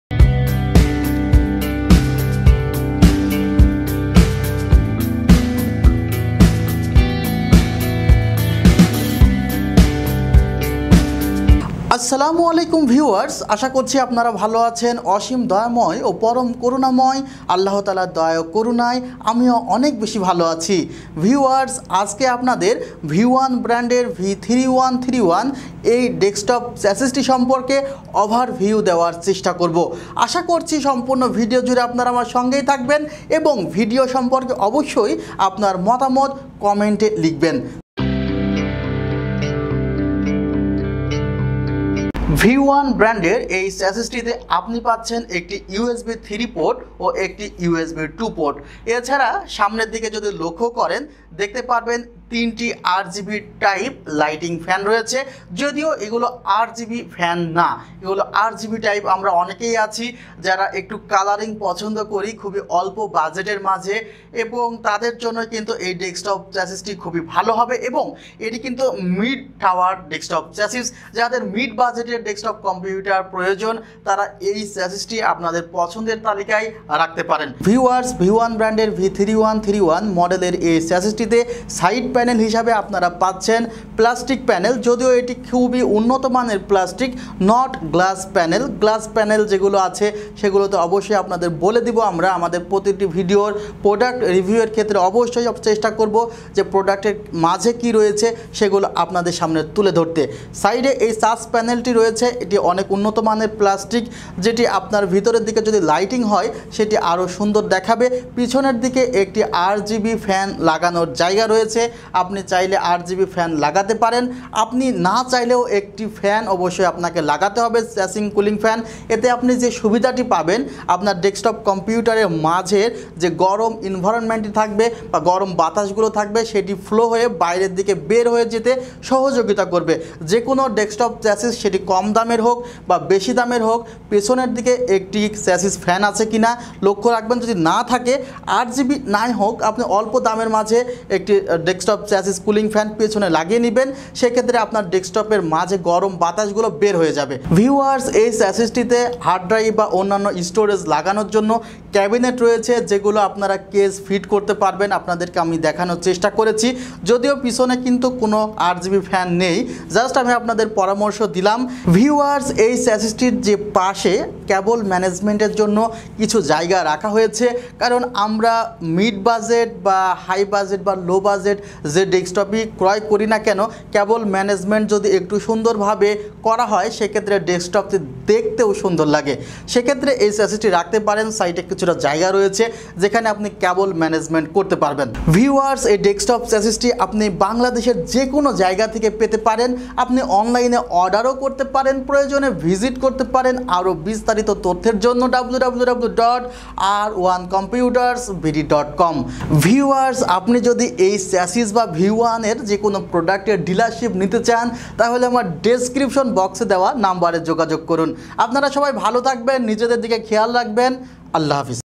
Best Desktop Case For Budget Users !!! View One V3131 Full Overview -Mehedi360BD
Mid Budget Desktop Casing Review in Bangla . View One V3131 Is The main Product to Review . I Hope You Enjoy This Video .